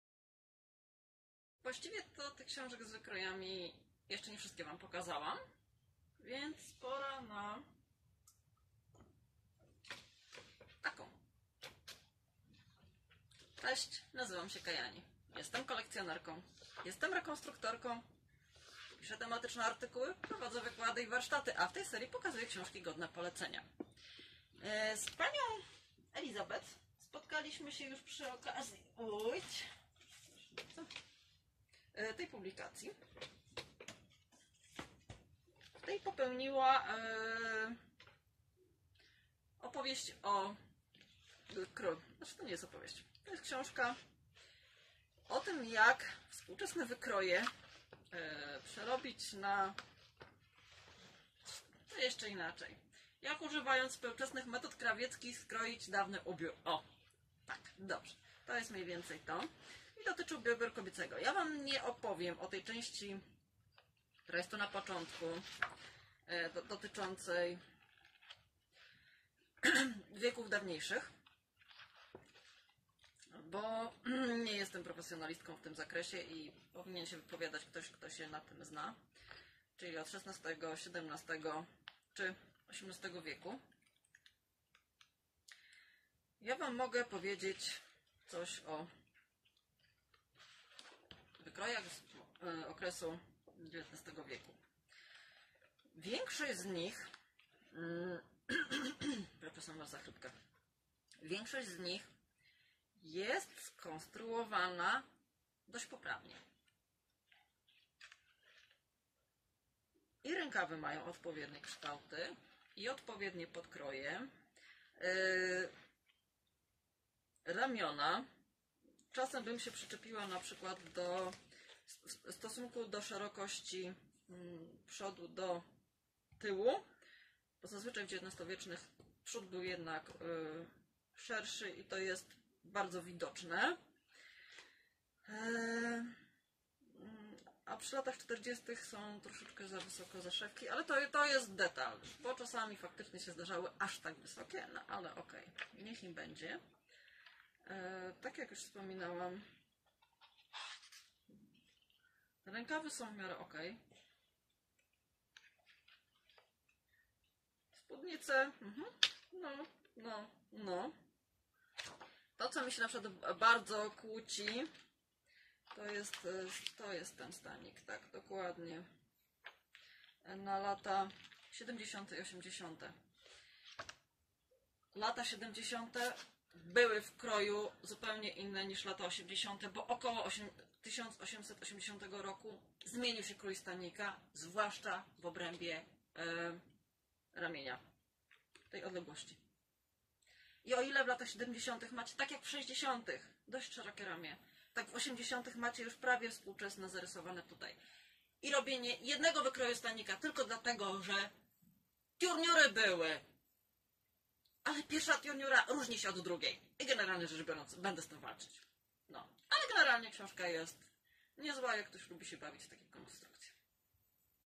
123. Elizabeth Friendship "Creating historical clothes Pattern Cutting from Tudor to Victorian Era"
To książka krawiecka, która pokazuje, jak używając współczesnych technik konstrukcyjnych wymodelować dawny ubiór.
   Nie powiem wam, czy wersja z Tudorami albo XVIII-wieczna jest poprawna - bo się na tym nie znam. Rozdział dotyczący XIX wieku trochę pobieżny, ale nie wymagajmy za wiele od książki, która ma za zadanie ogólnie pokazać krój, a nie wszystkie możliwe warianty.
BŁĄD, który mnie do tej książki zniechęca, to to błędne przekonanie, że suknie z obu okresów turniurowych krojono jednakowo. Nie. Nie. Nie.